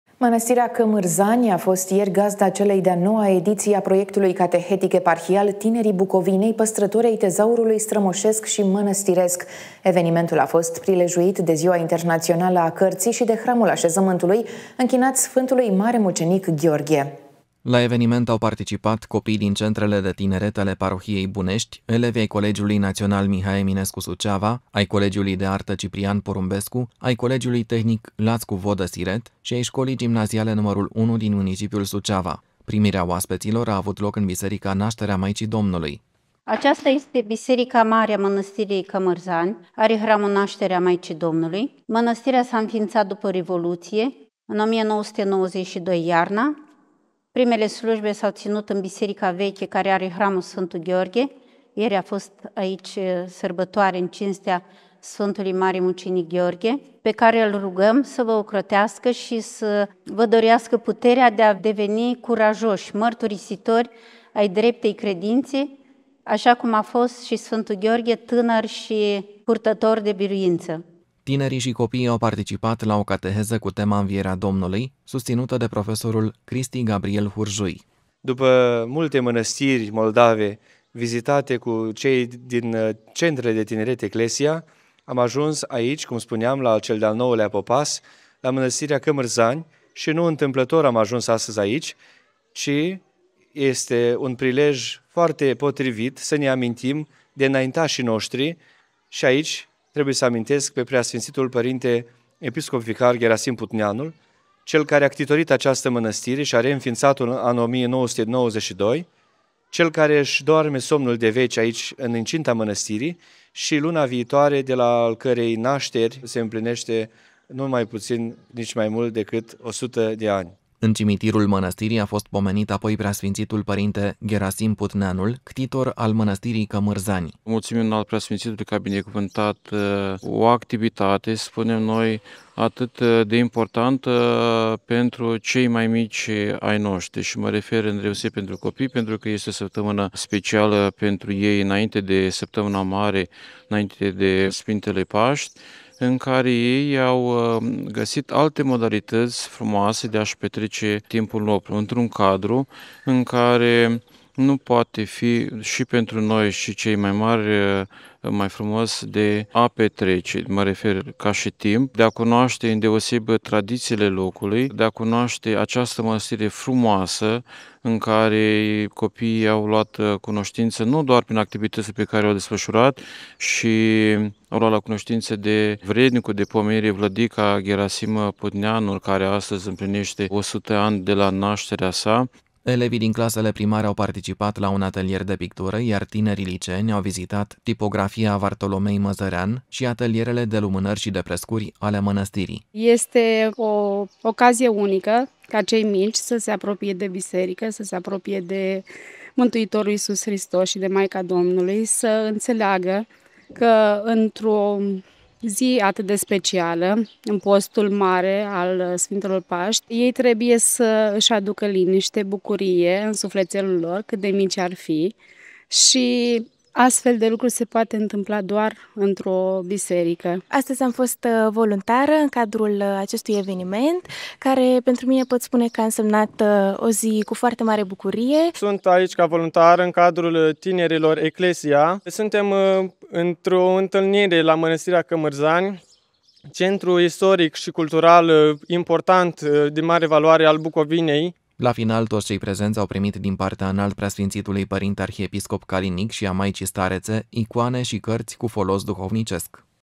Ediția a IX-a a Proiectului „Tinerii Bucovinei, păstrători ai tezaurului strămoșesc și mănăstiresc”
Mănăstirea Cămârzani a fost ieri gazda celei de a IX-a ediții a Proiectului Catehetic Eparhial „Tinerii Bucovinei, păstrători ai tezaurului strămoșesc și mănăstiresc”. Evenimentul a fost prilejuit de Ziua Internațională a Cărții și de hramul așezământului, închinat Sfântului Mare Mucenic Gheorghe.